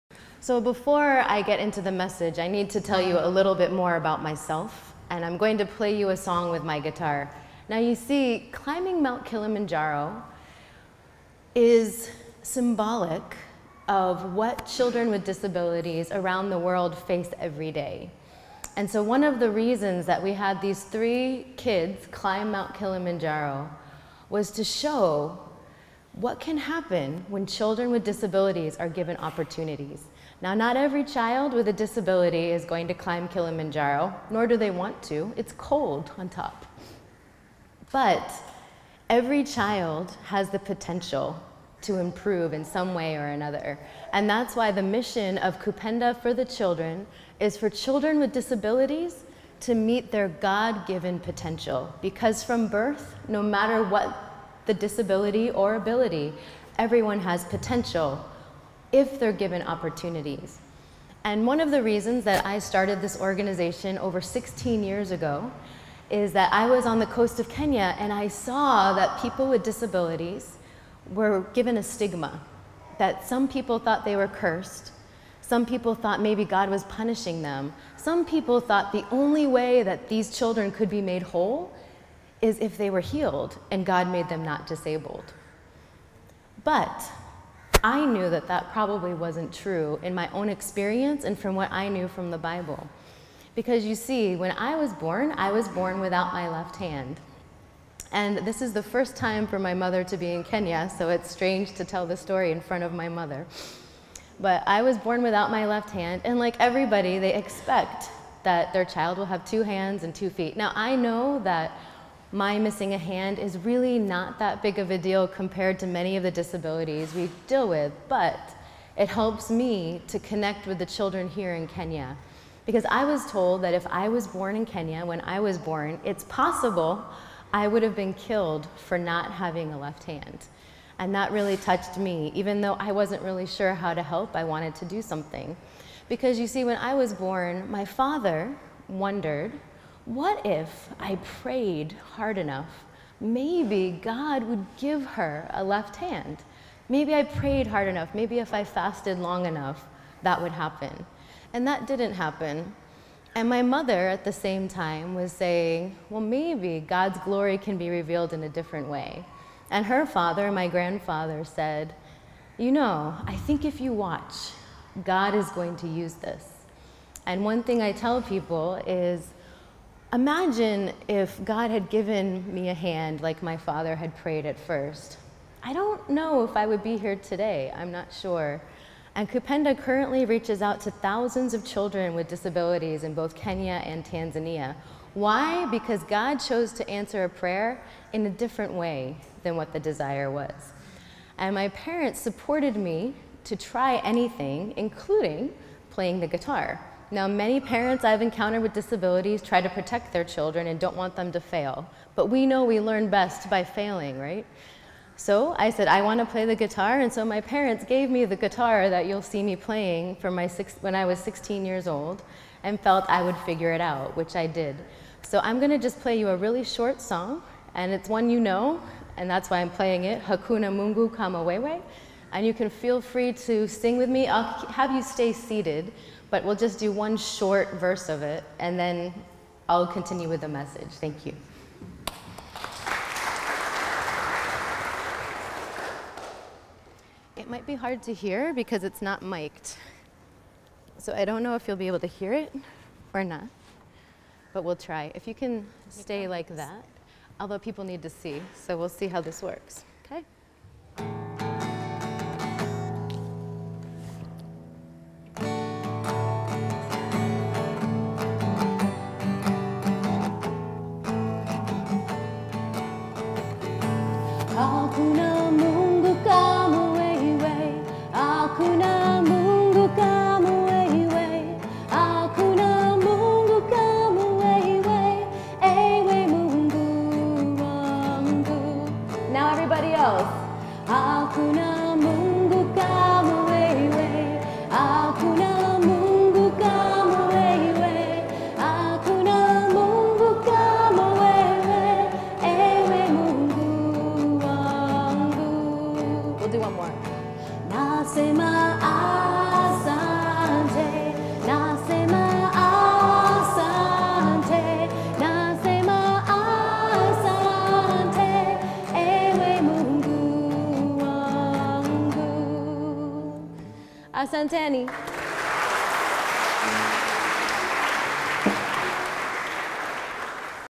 Cynthia Bauer Personal Disability Experience and Guitar Playing in Kenya
Kupenda's co-founder and executive director shares her personal experiences of living with a disability at an Anglican Church in Mombasa, Kenya. She also plays a local Swahili song with one hand to show that disability is not in ability.